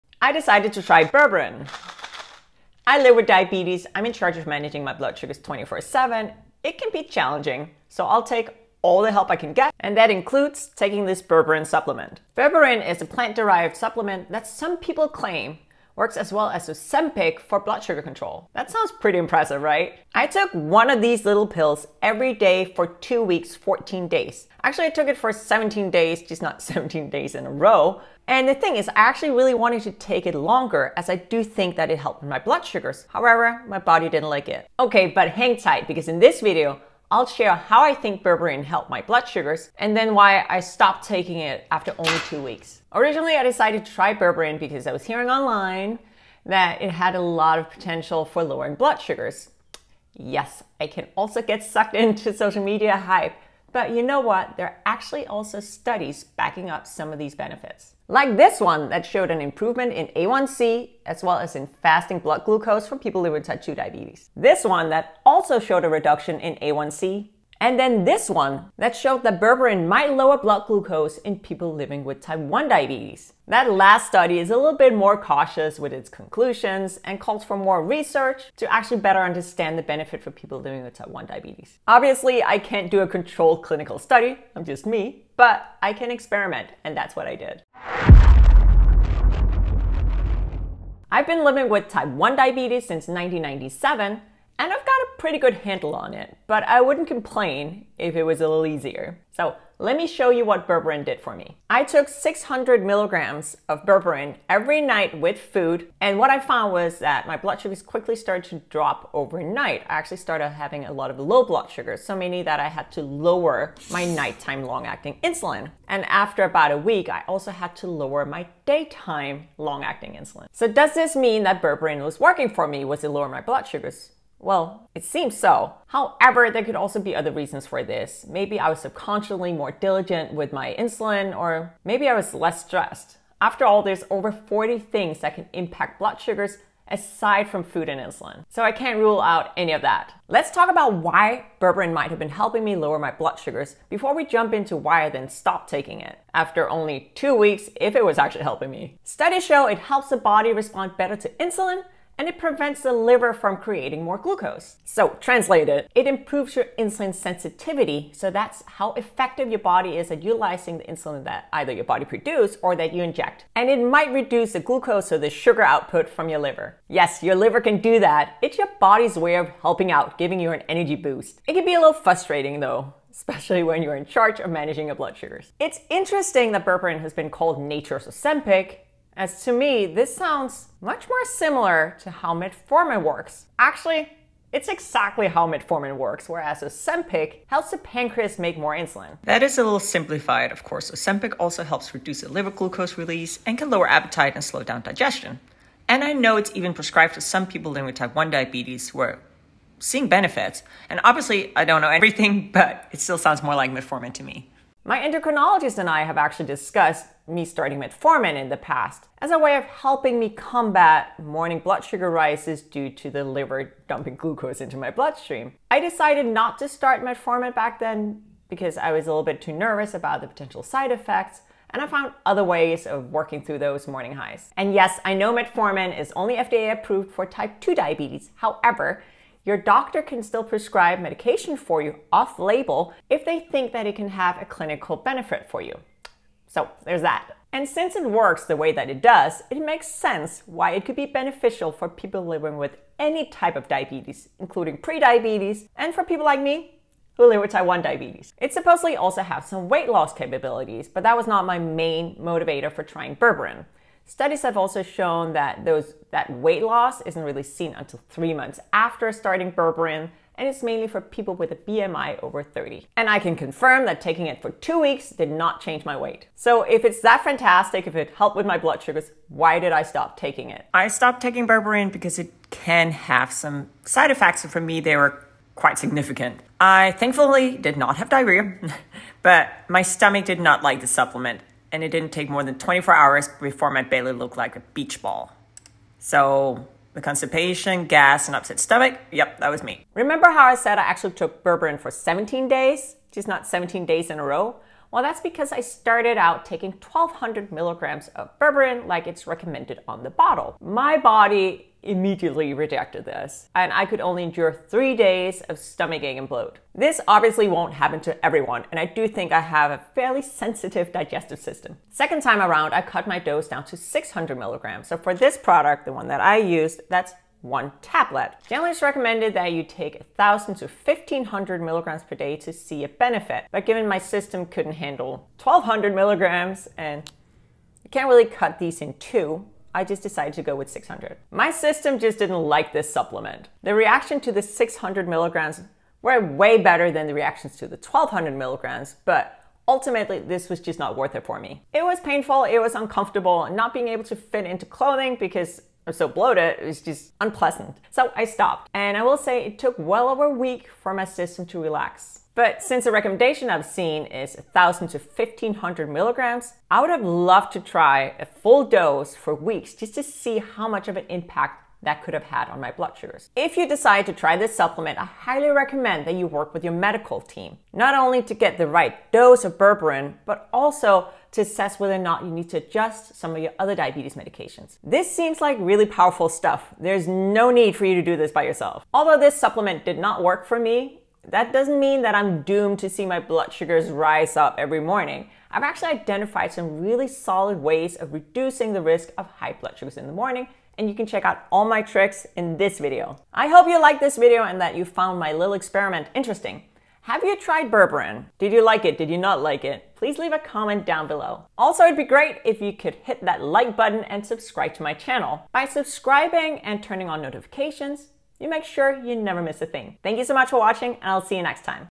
I Took Berberine for 14 Days – It lowered my blood sugars BUT…
Can Berberine help lower blood sugars? Check out my Berberine diabetes experiment.

This is the Berberine supplement I tried

This is my personal experience using Berberine. It’s a supplement and it might work very differently for you, or not at all.

Video Chapters:
0:00 Intro
0:53 Studies supporting Berberine blood sugar claims
02:20 How does Berberine work
04:30 Berberine side effects